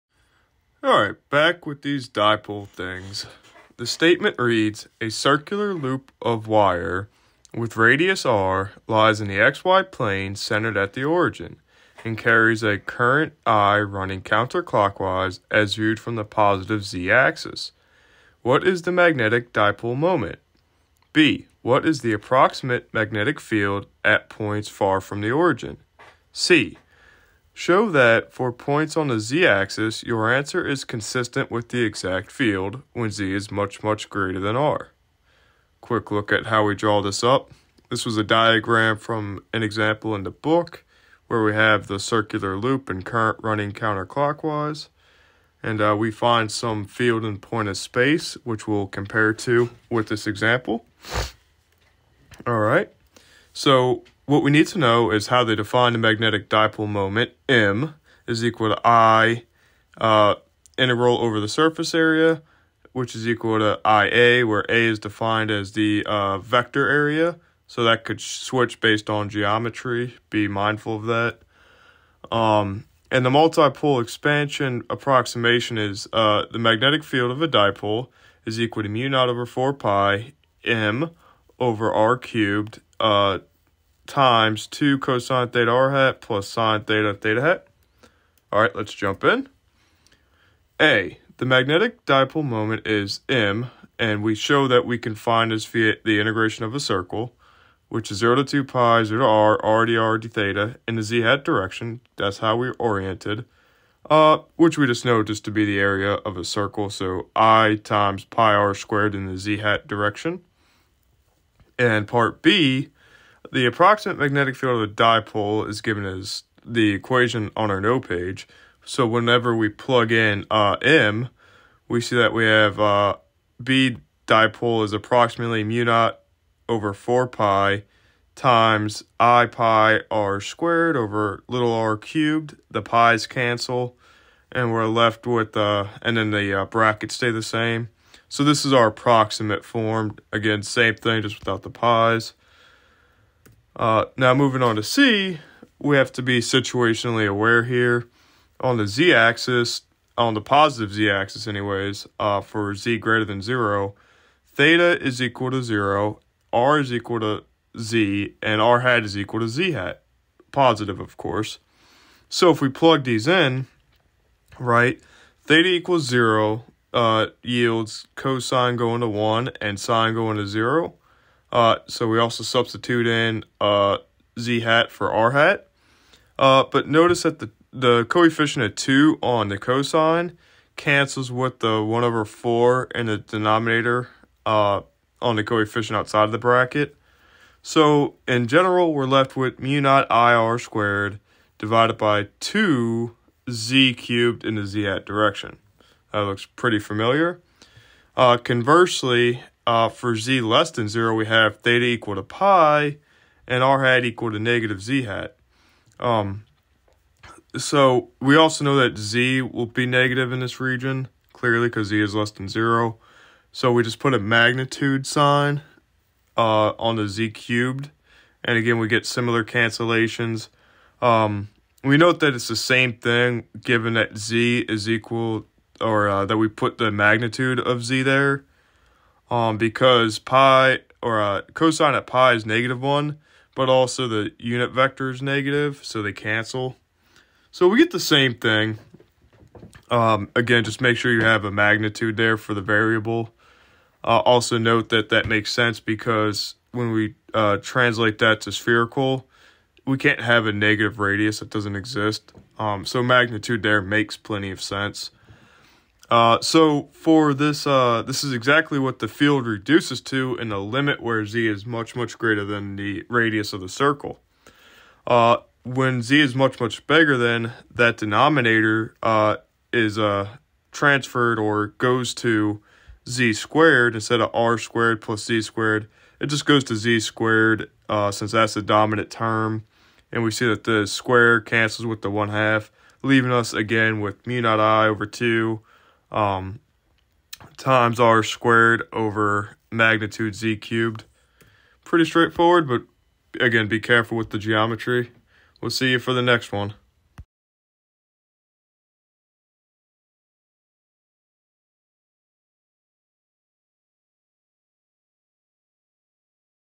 Problem 5.35 - Magnetic Vector Potential, Multipole Expansion: Introduction to Electrodynamics
Isn't it lovely how these things work out? Though hindsight is 20/20, being able to see that applying and approximation like this can lead to fruitful and accurate results is pretty nice! Perspective can speed up work too; use your tools wisely.